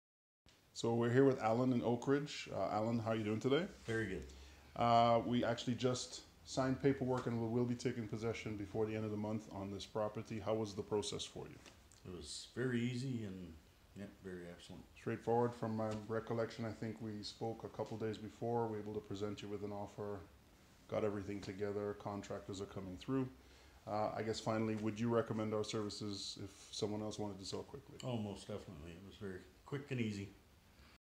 Quick, private sale of home in Oakridge - Allan M. (CalgaryHomeSale testimonial)
Allan and Annette are seasoned investors who wanted to discretely sell a property to downsize a family member, and purchase another home.  We were able to get them the cash 'quick and easy' at a price that worked for them and at a date of their choosing.